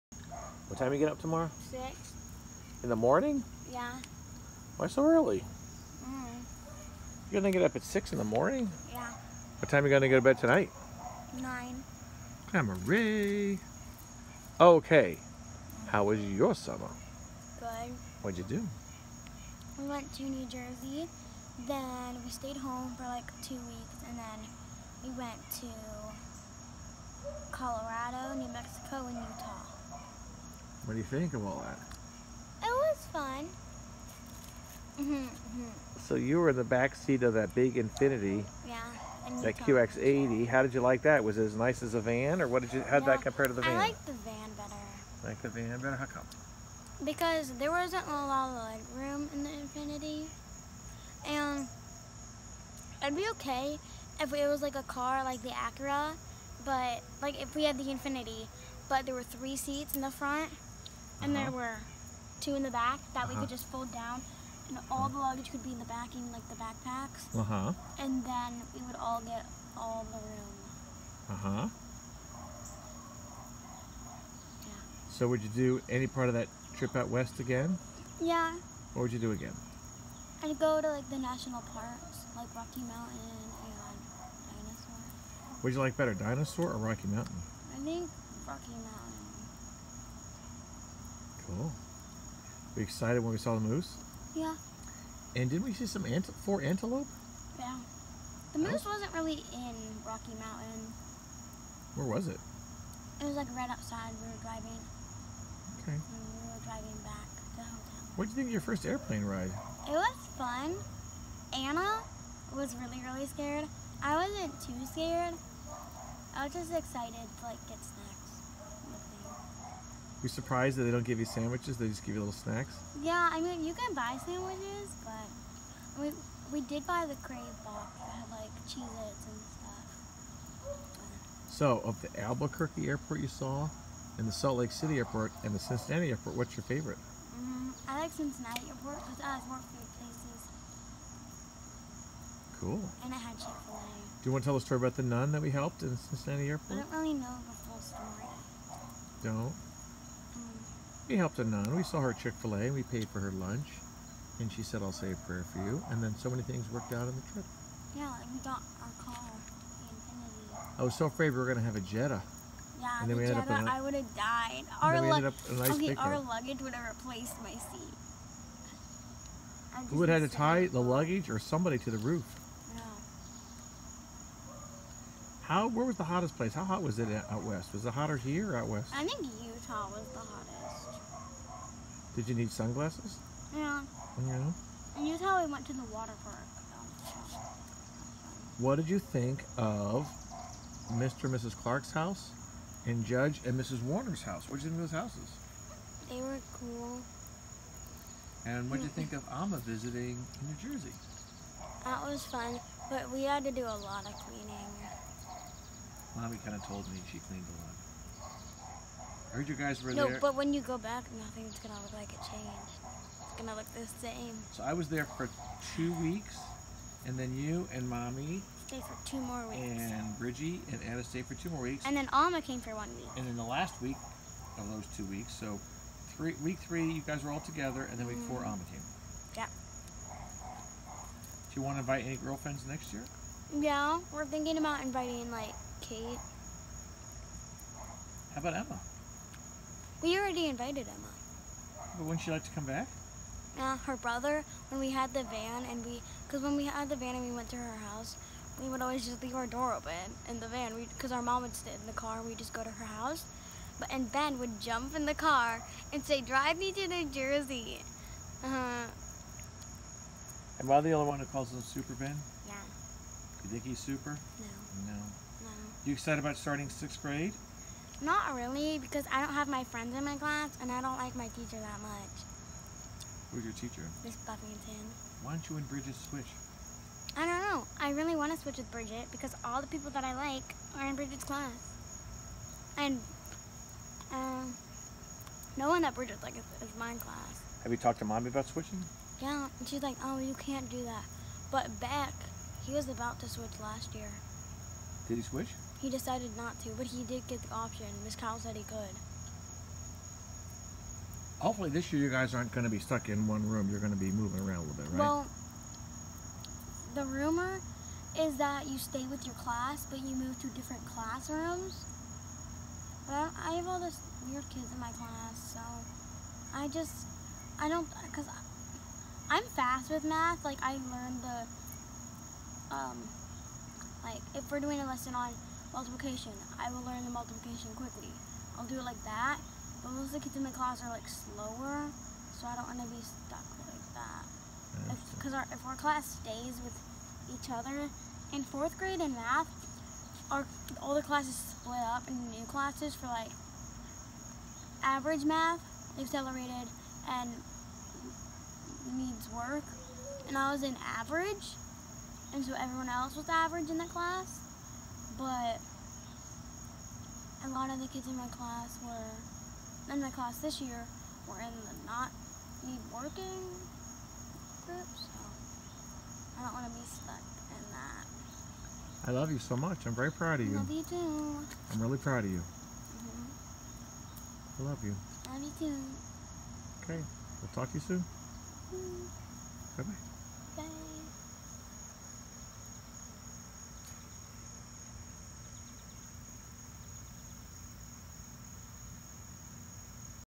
Clare reflects on Summer 2018 - just before she enters of the sixth grade – August 2018
Clare reflects on Summer 2018 - just before she enters the sixth grade – August 2018